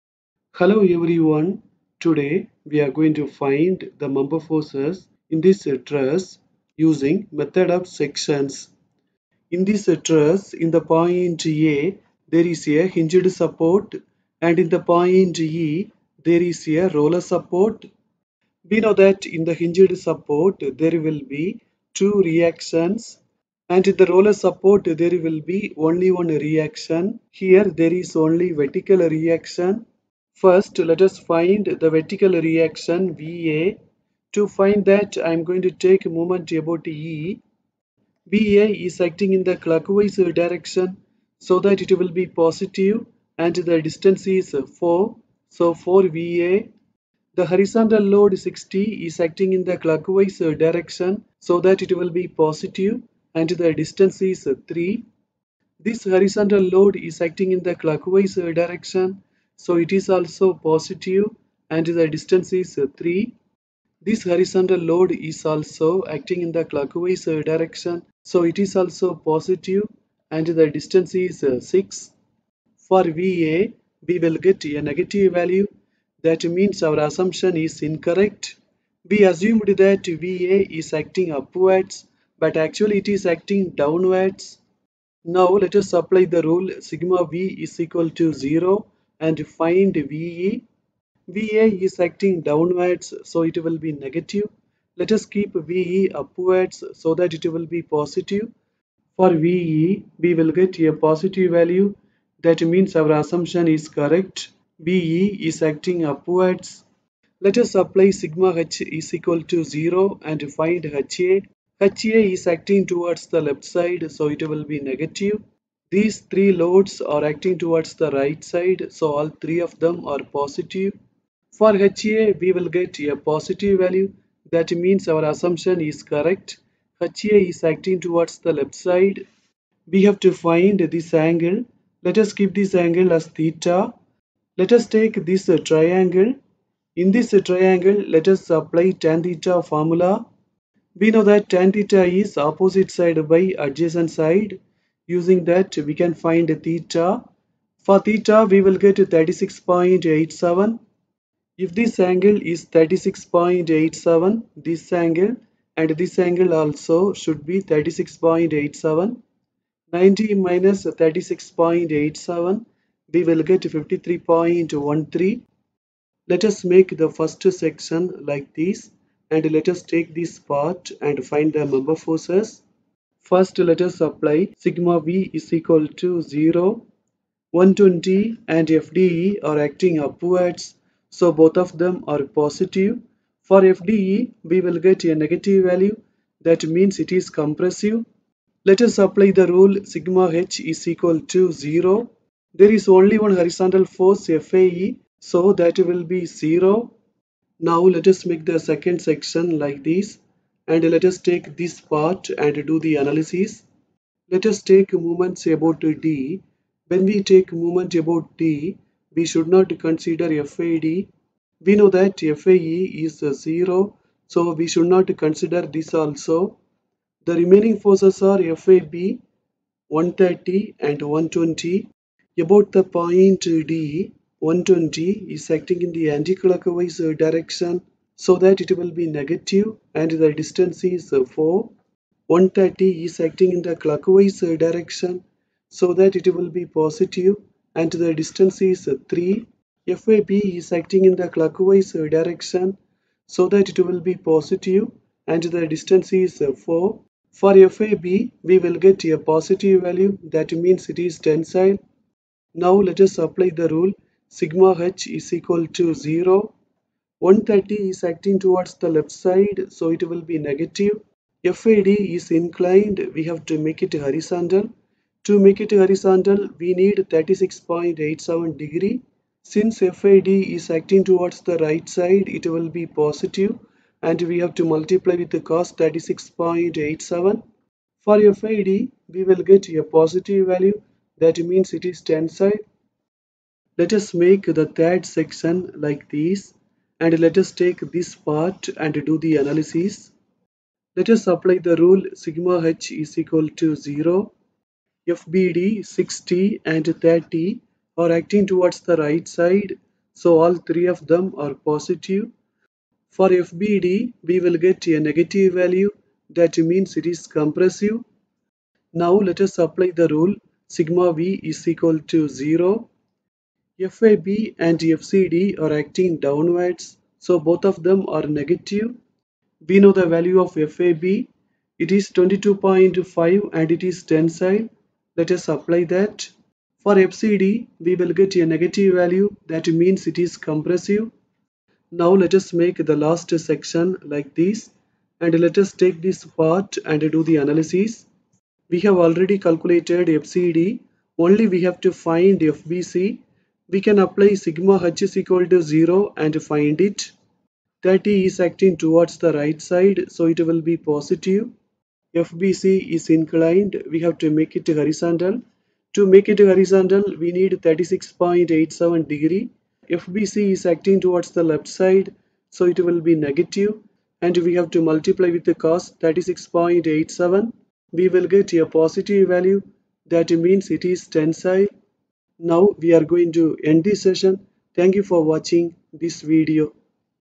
Analysis of Trusses by Methods of Sections - Problem 9
Same truss has been analysed by method of joints,